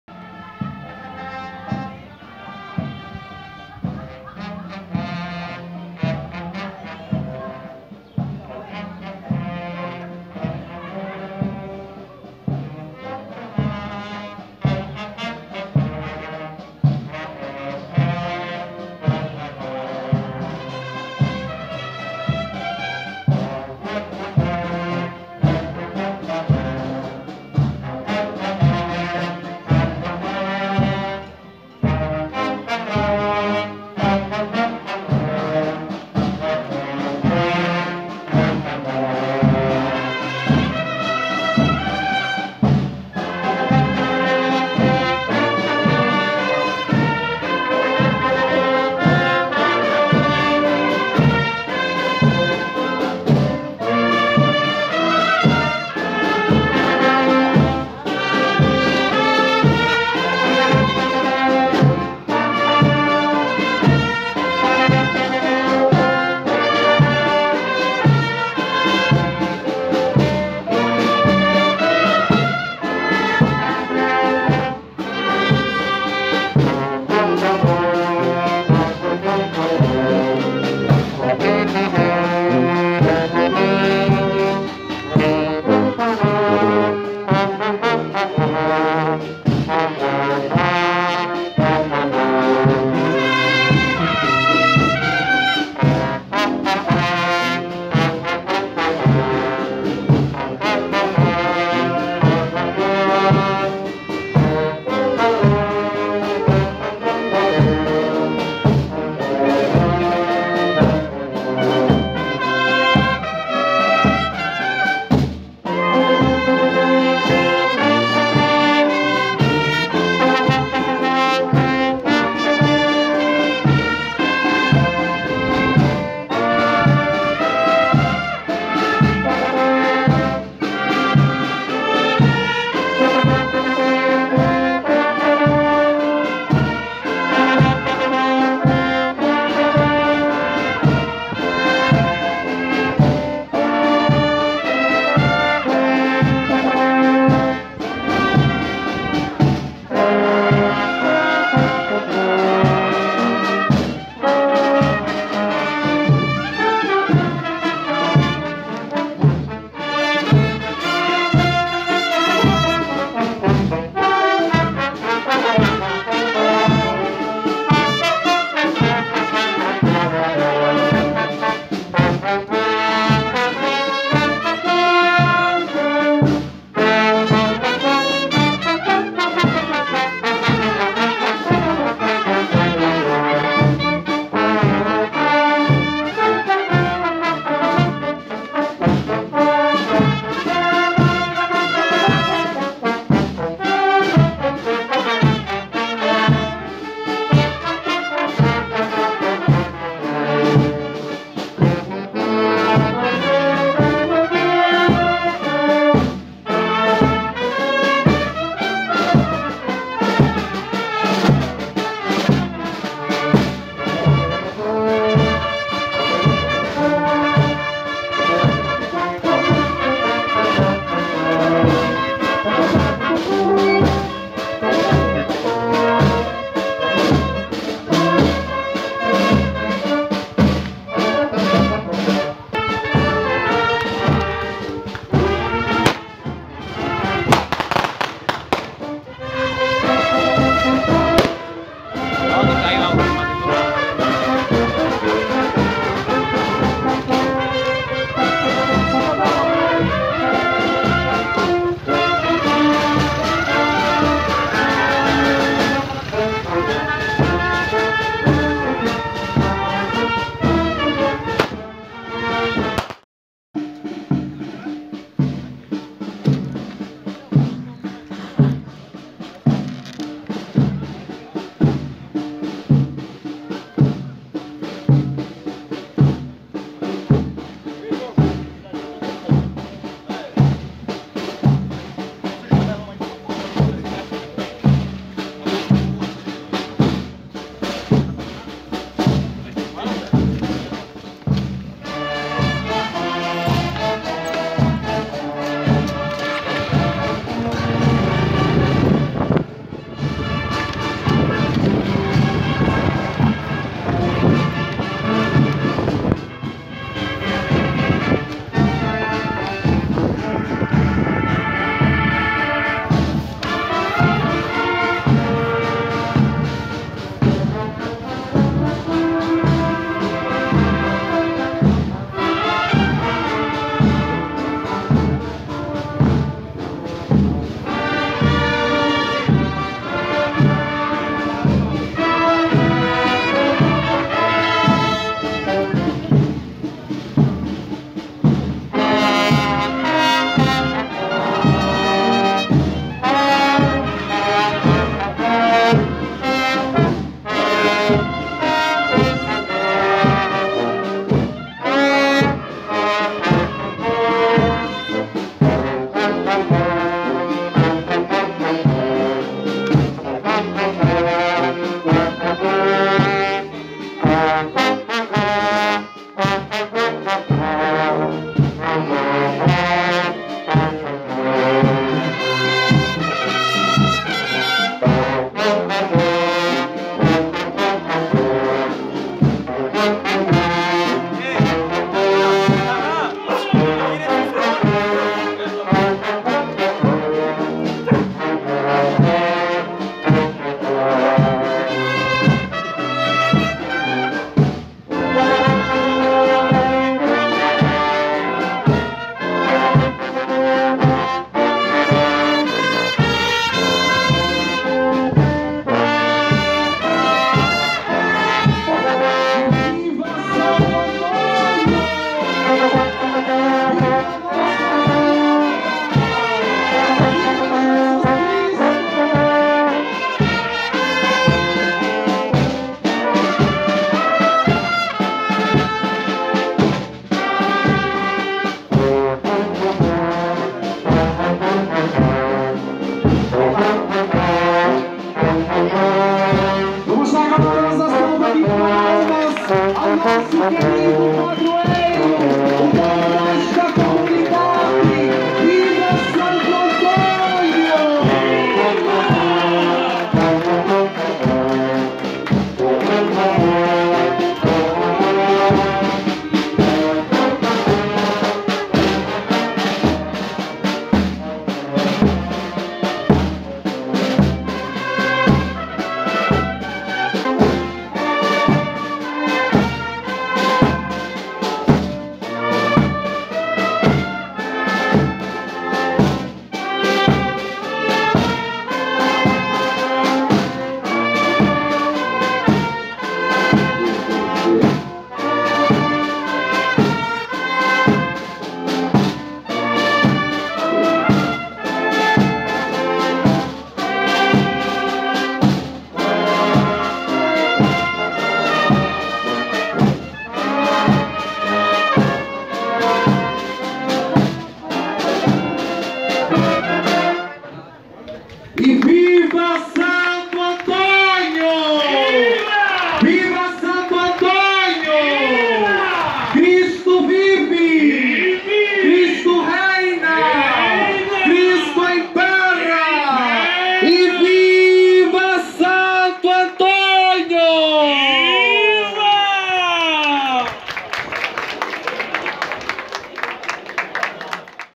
PENEDO/AL TEM: Alvorada de Sto Antônio com a Banda do Monte Pio.
Alvorada com a Banda do Monte Pio de Penedo pelas principais ruas do Bairro Vermelho em homenagem ao seu Padroeiro.